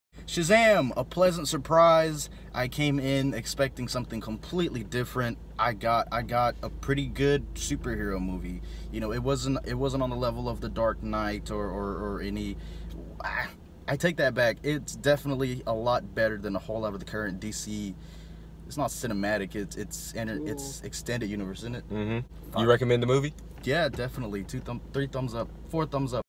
World's Shortest Shazam! Movie Review
Short on time but wanna know if Shazam! is worth seeing, this the video for you. Straight to the answer spoiler free review.